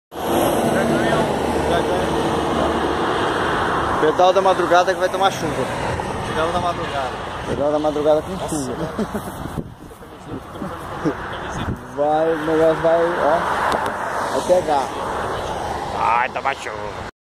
PedalAraraquara VD2 - Selfie no Pedagio
PedalAraraquara em mais um passeio de bike até o Pedágio! Dia muito bonito, saímos cedo até o pedágio e voltamos pela chácara do Nelson. Um ótimo ambiente para encontrar amigos e dar boas risadas em mais uma aventura do PedalAraraquara!